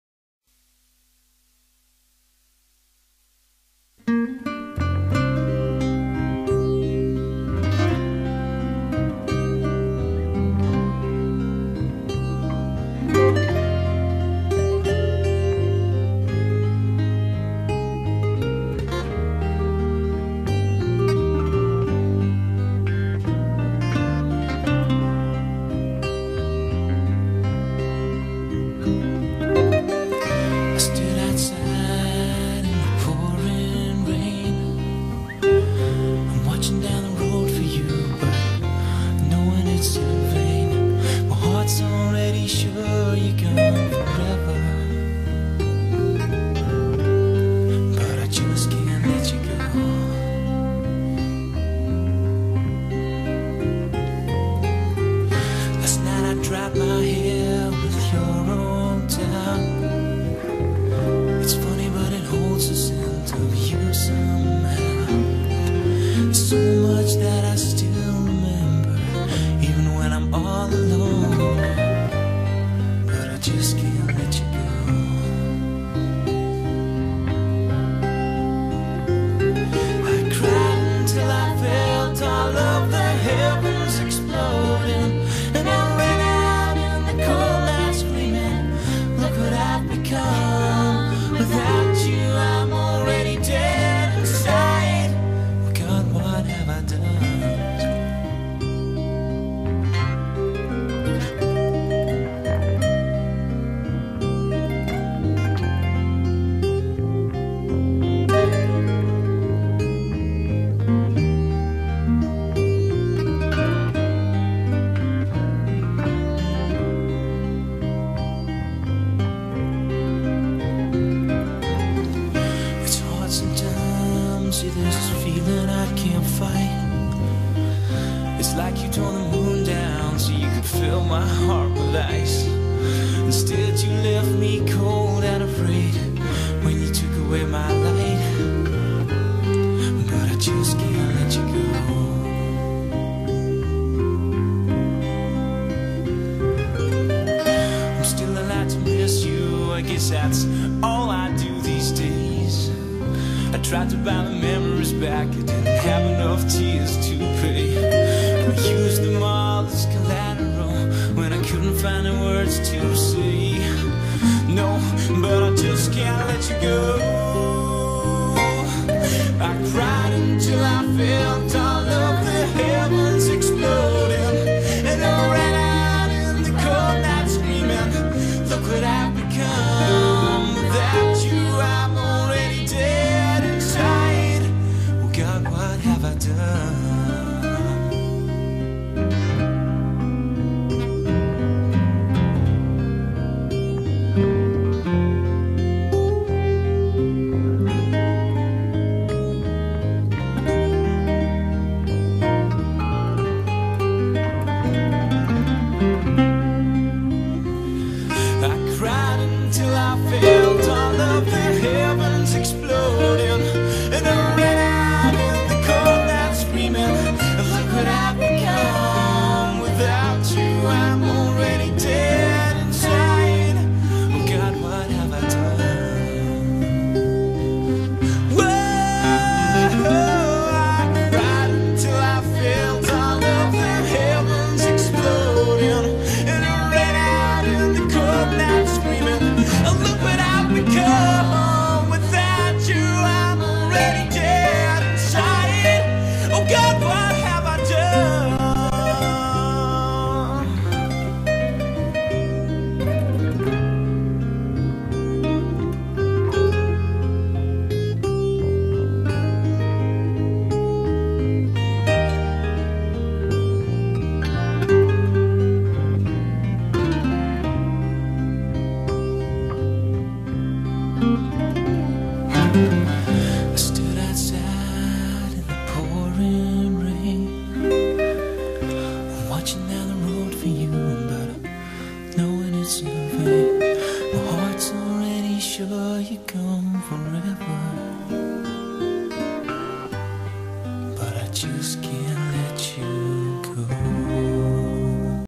James "Hetfield" Cresswell. 1992-2012
a tribute to a good friend - Jimmy.  taken from us way too young...
we miss ya buddy.

I stood outside in the pouring rain,
watching down the road for you, knowing it's in vain
My heart's already sure you're gone forever,
but I just can't let you go.

inside I dried my hair, with your old towel
it's funny but it holds the scent of you somehow.
there's so much that I still remember even when I'm all alone

I cried until I felt all of the heavens exploding
and I ran out in the cold night screaming "Look what I've become"
without you I'm already dead inside
Oh god what have I done..?

It's hard sometimes, see there's this feeling I can't fight
it's like you tore the moon down so you could fill my heart with ice
instead you left me cold and afraid when you took away my light

I'm still allowed to miss you - I guess that's all I do these days
I tried to buy the memories back, didn't have enough tears to pay
I used them all as collateral when I couldn't find the words to say.